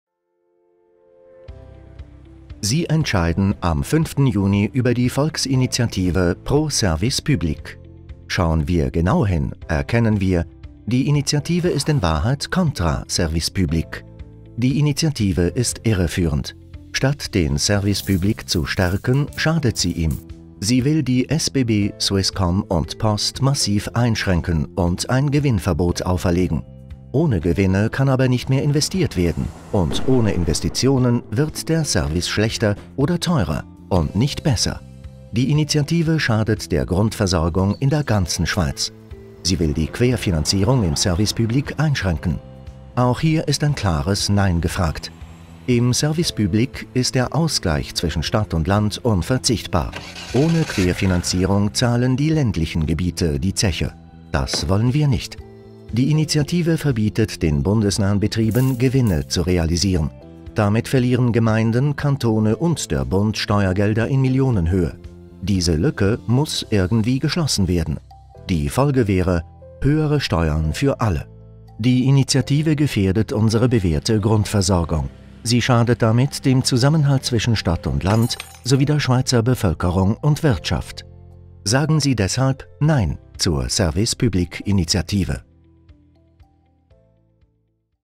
Nein zur schädlichen und irreführenden Initiative «Pro Service public»
Die Schweizerische Arbeitsgemeinschaft für die Berggebiete (SAB) und der Schweizerische Gemeindeverband (SGV) sind alarmiert über die Volksinitiative «Pro Service public»: Sie ist eine Mogelpackung und ein Etikettenschwindel. Die Initiative gefährdet die gute flächendeckende Grundversorgung und setzt das Schweizer Erfolgsmodell aufs Spiel. Deshalb setzen wir uns ein für ein Nein zur schädlichen Service-public-Initiative am 5. Juni 2016!